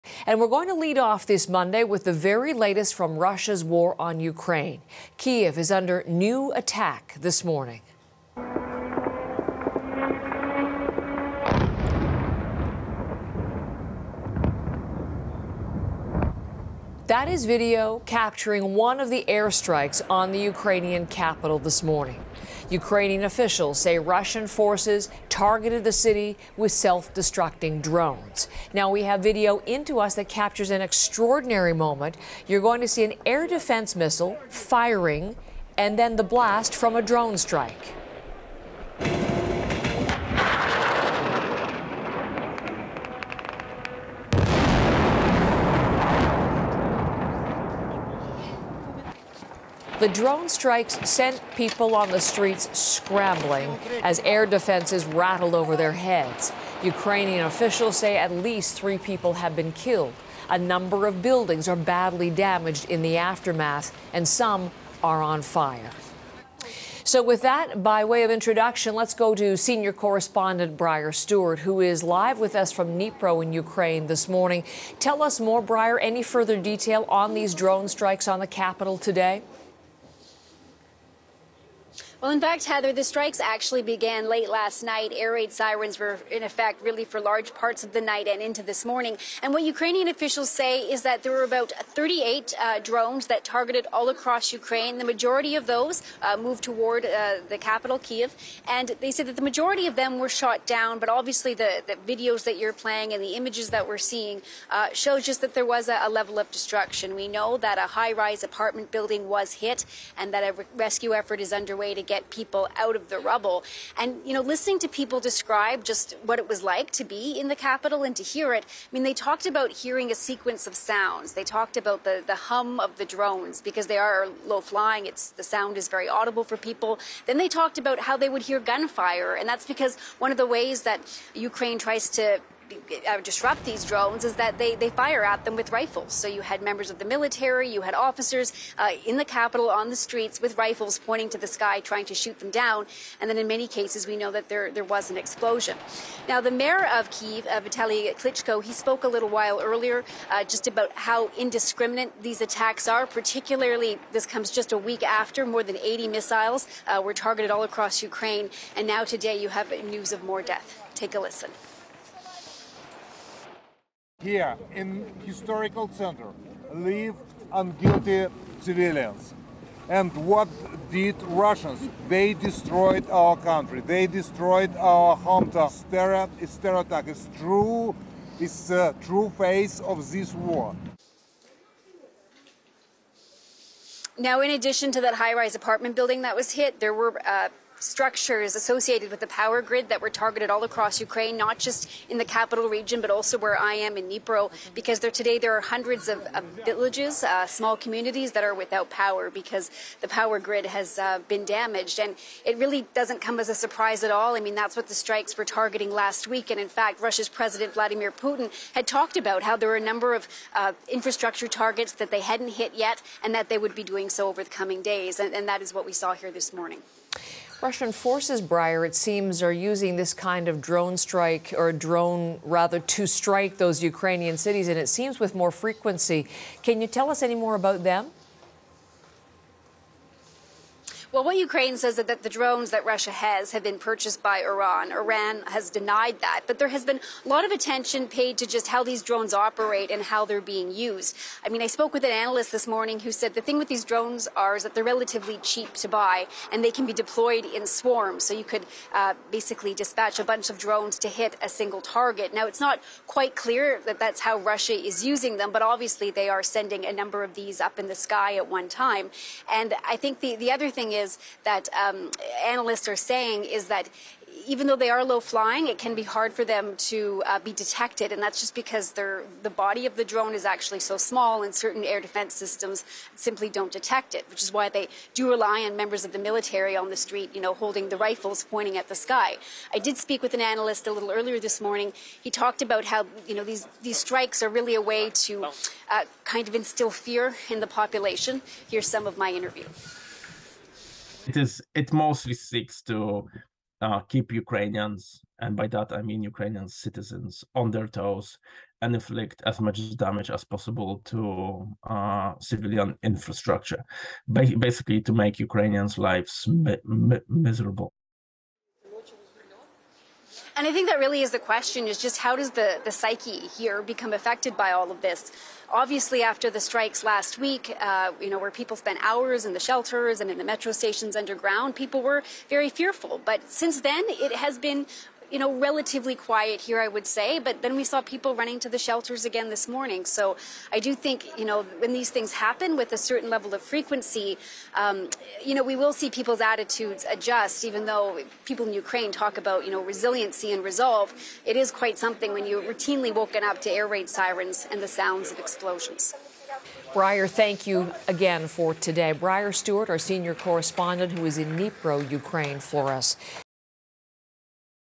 Russia targets Kyiv with self-destructing drone strikes
Waves of explosive-laden suicide drones struck Ukraine's capital as families were preparing to start their week, setting buildings ablaze and sending people scurrying to shelters.